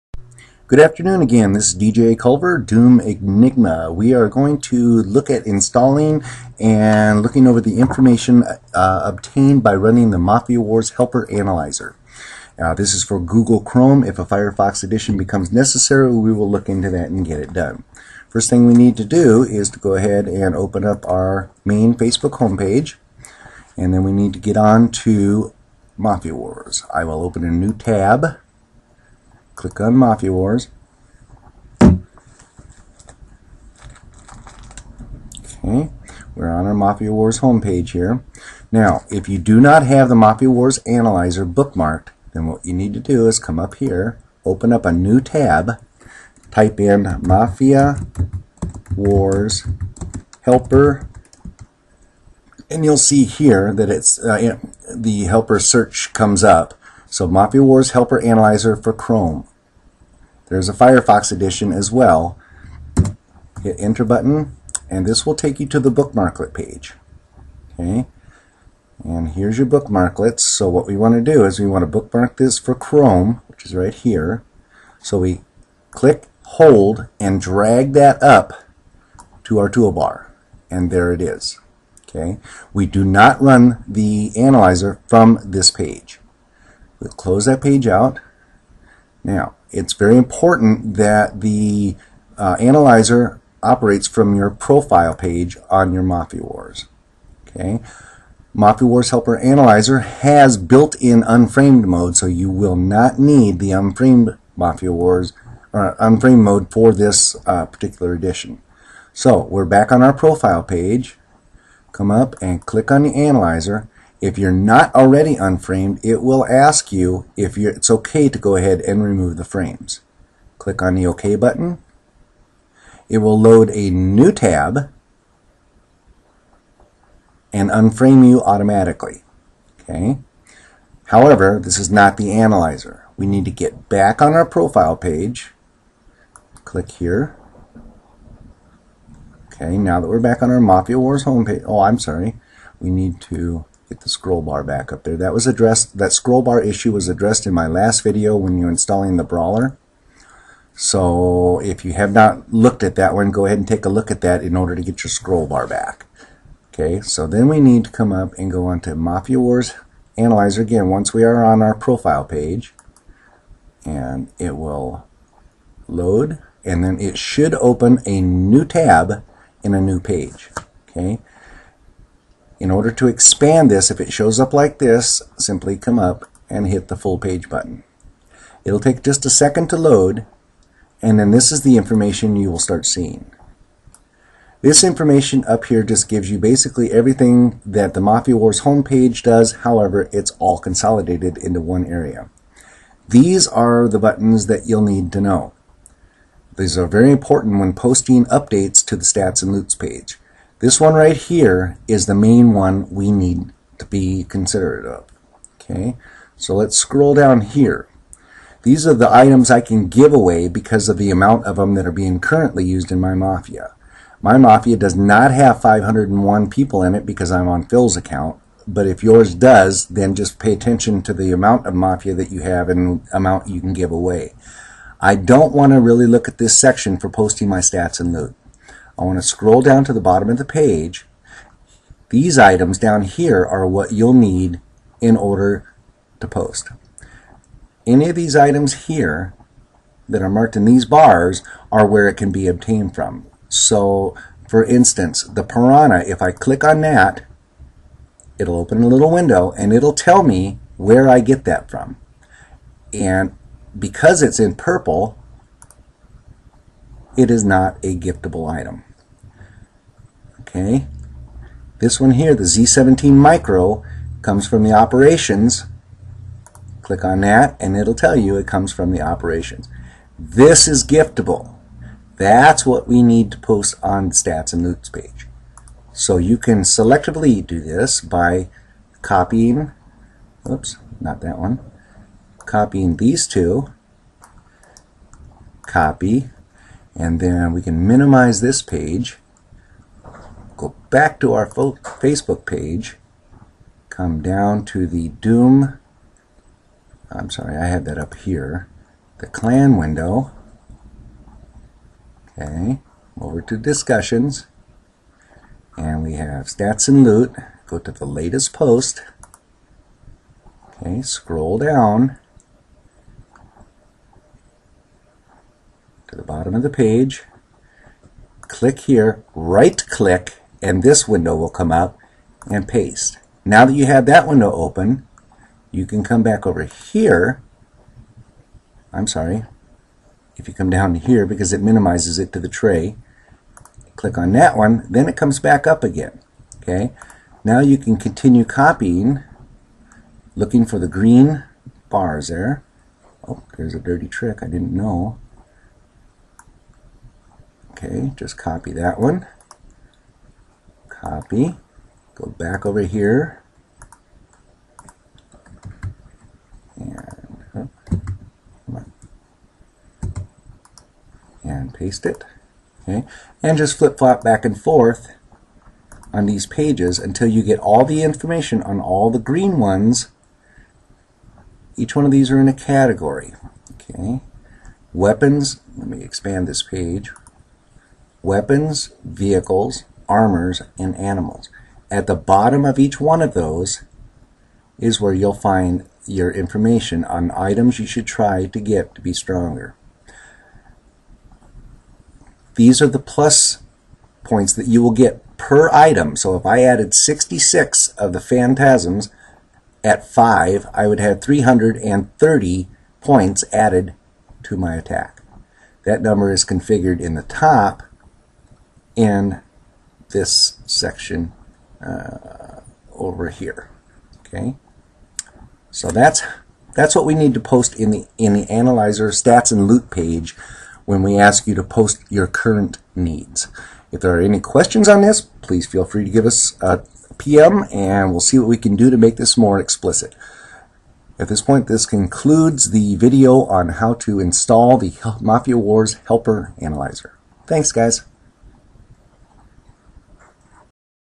MWHelper Analyzer.wmv
How to run the MW Helper analyzer in an unframed mode.